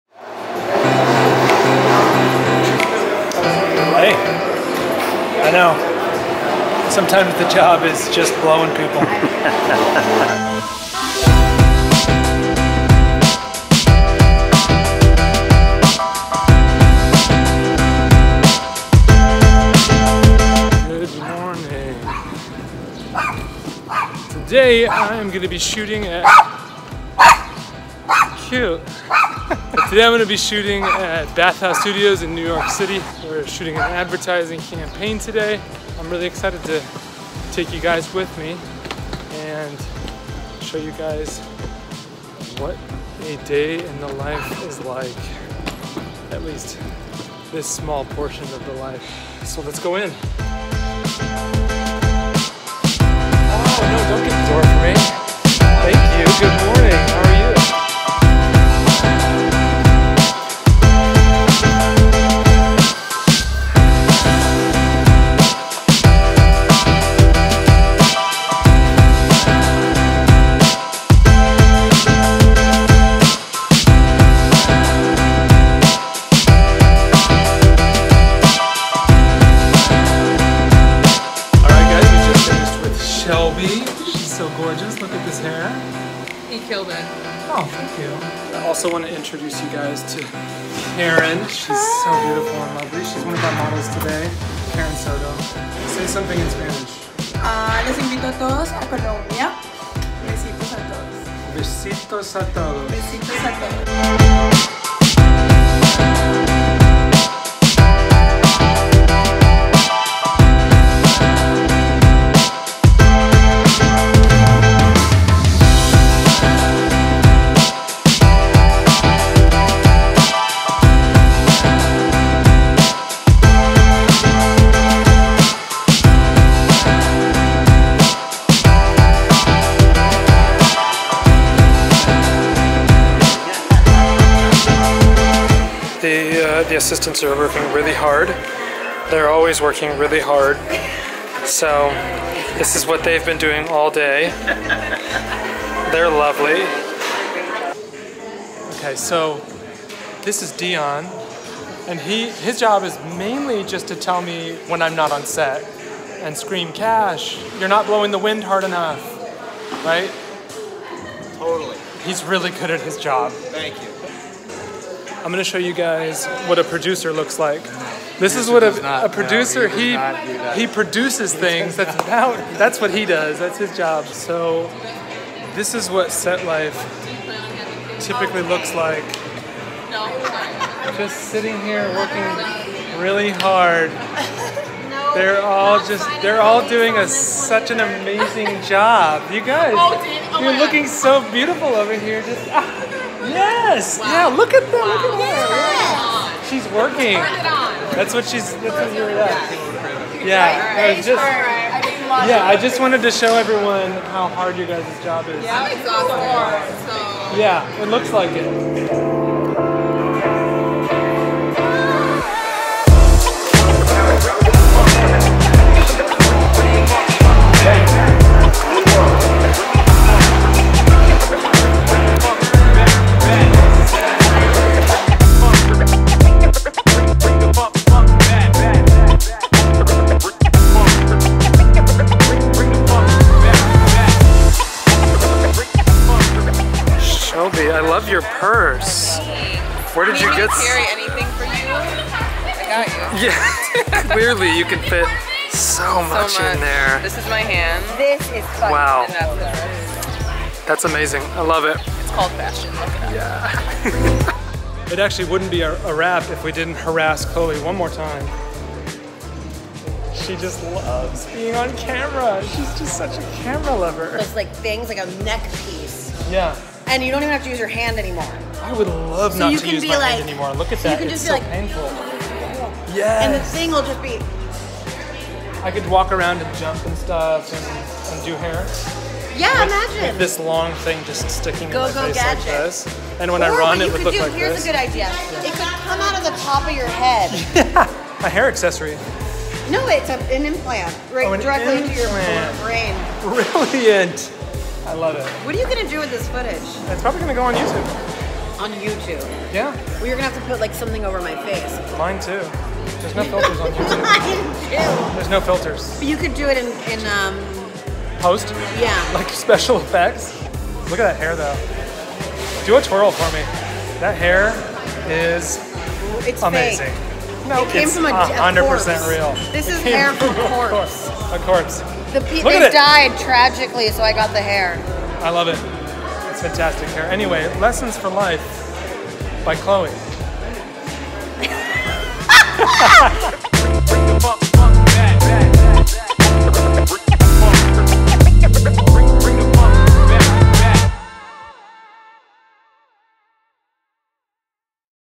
HAIR PHOTOSHOOT - Hair Stylist Photoshoot Breakdown
A fashion photoshoot in New York City used to make me nervous. As a hair stylist play a particular role on a photoshoot. You never know what could happen! But after 11 years of being on set shooting campaigns and editorial hair for major brands and magazines, I'm excited to show you what a day behind the scenes on a photo shoot looks like.